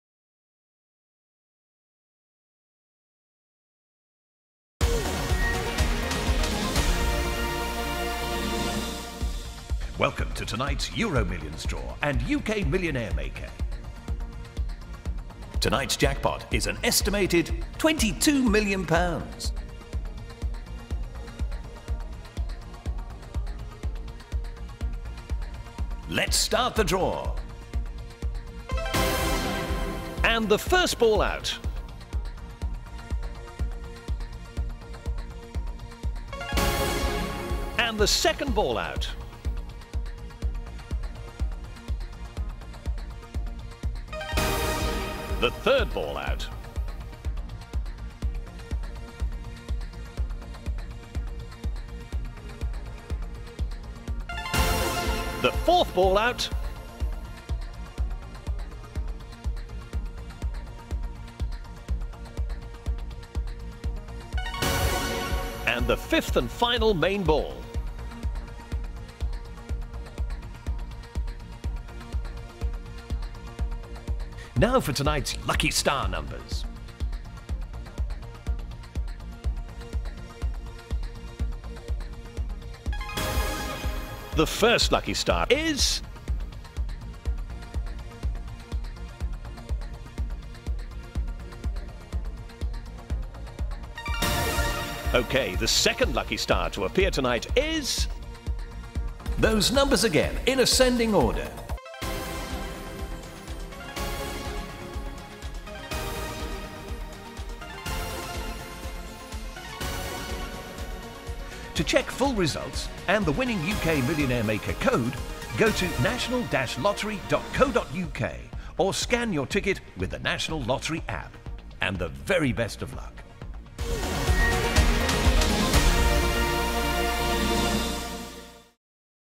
The National Lottery ‘EuroMillions’ draw results from Tuesday 18th June 2019
The National Lottery EuroMillions draw results from Tuesday 18th June 2019. Rules and Procedures (and Account Terms if playing online) apply. Players must be 16+ and physically located in the UK or IoM.